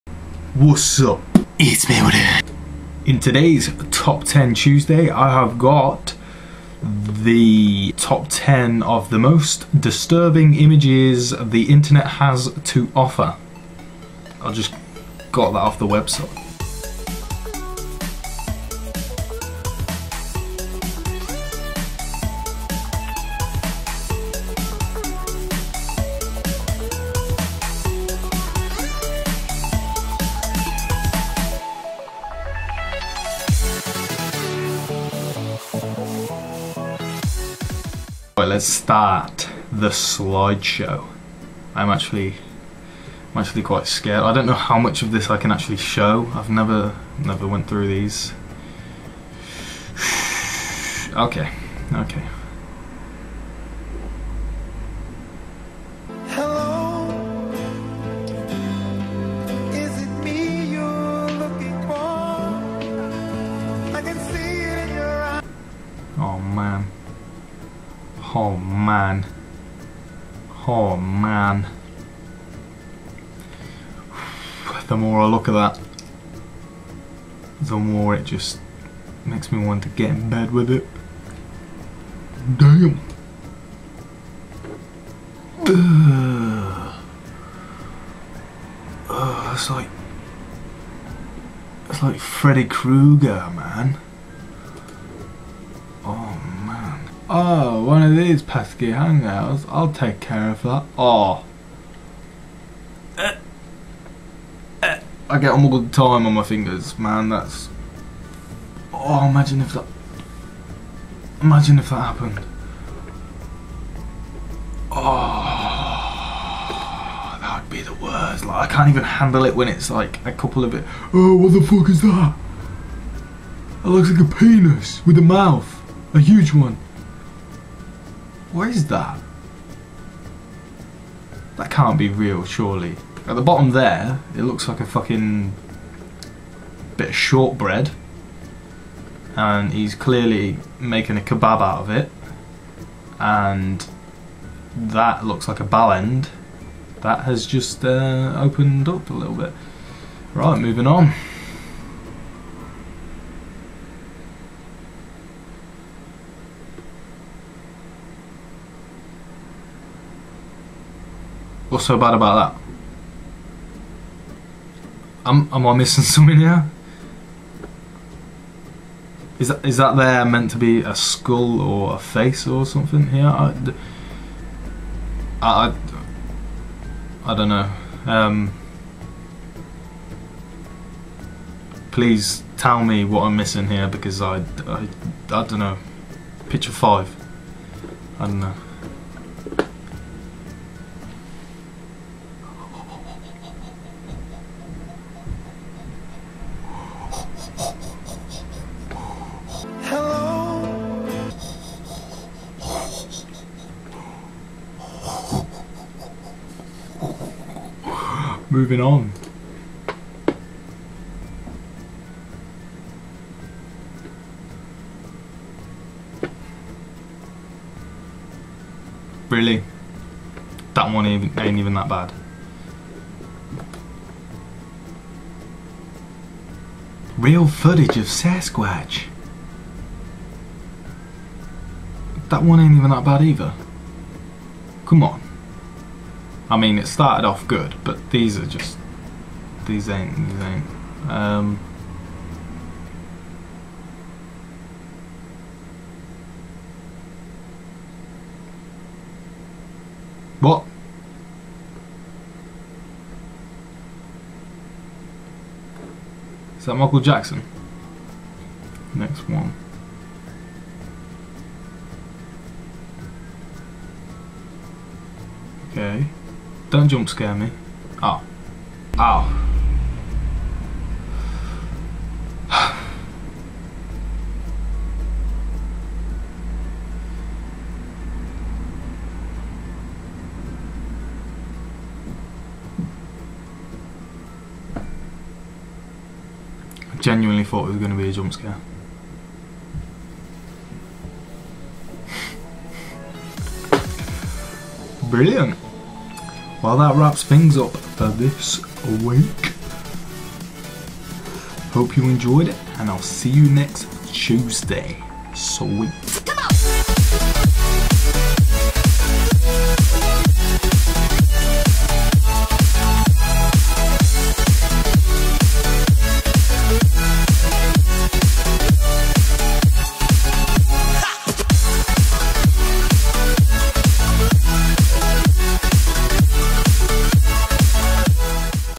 Top Ten - Most disturbing images on the web
This weeks TTT is the most disturbing images on the internet. well... I found a site claiming to be this so lets have a look.

let me know what picture 5 and 9 is all about?

►►JOIN US◄◄
AND

I would really appreciate the support!